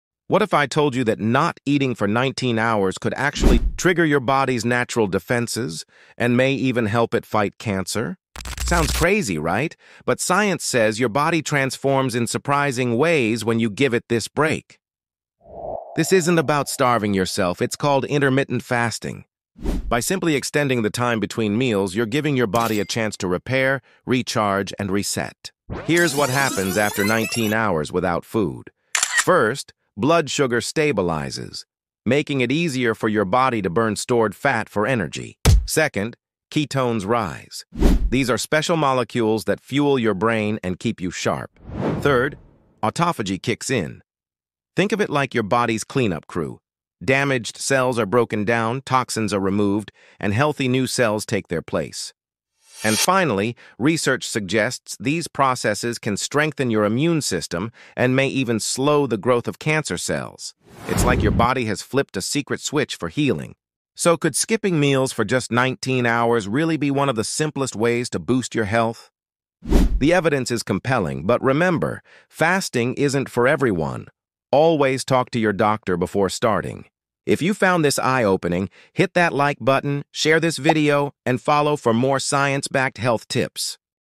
The Hidden Power of Fasting Your Doctor Didn’t Tell You!
What actually happens inside your body when you don’t eat for 19 hours?
In this video, we break down the hidden science of intermittent fasting — from fat-burning and ketones to autophagy and immune system activation. You’ll discover how a simple 19-hour fast may support your body’s natural healing processes, including mechanisms that may help your body fight cancer.